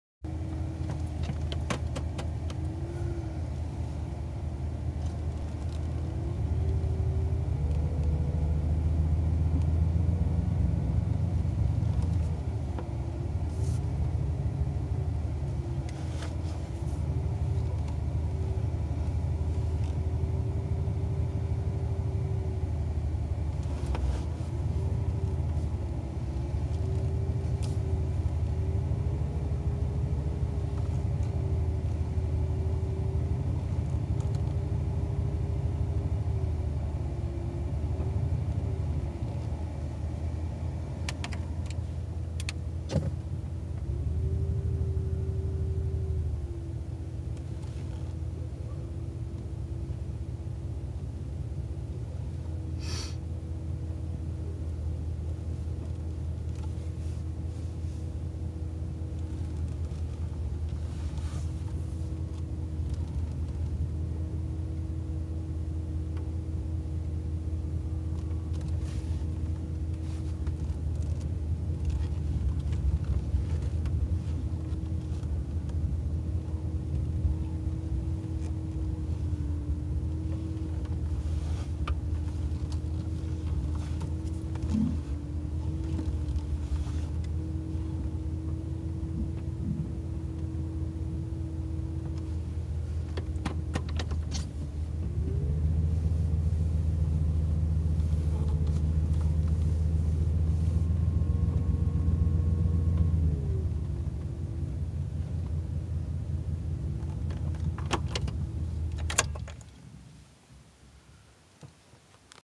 Moving large bales 2011 (8)
View from inside the cab as I steer the second bale into it's position in the feeder.